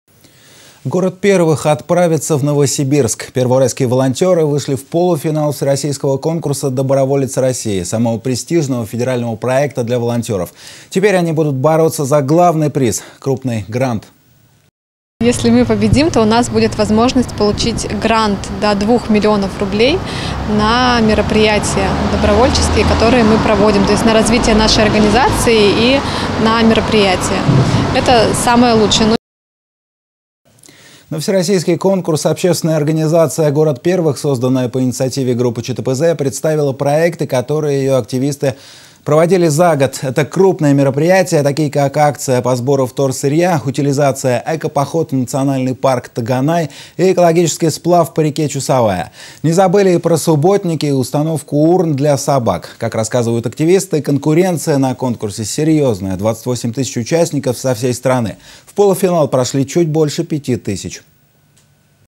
«Город первых» вышел в полуфинал всероссийского конкурса
Город первых отправится в Новосибирск. Первоуральские волонтеры вышли в полуфинал всероссийского конкурса Доброволец России, самого престижного федерального проекта для волонтёров. Теперь они будут бороться за главный приз - крупный грант.

Вера ИВАНИЩЕВА, активист общественного движения "Город первых": «Если мы победим, то у нас будет возможность получить грант до 2 миллионов рублей на мероприятия добровольческие, которые мы проводим. То есть, на развитие нашей организации и на мероприятия. Это самое лучшее».

На всероссийский конкурс общественная организация "Город первых", созданная по инициативе Группы ЧТПЗ, представила проекты, которые её активисты проводили за год. Это крупные мероприятия - такие, как акция по сбору вторсырья - "Утилизация", экопоход в национальный парк "Таганай" и экологический сплав по реке Чусовая. Не забыли и про субботники и установку урн для собак. Как рассказывают активисты, конкуренция на конкурсе серьезная - 28 тысяч участников со всей страны. В полуфинал прошли чуть больше пяти тысяч.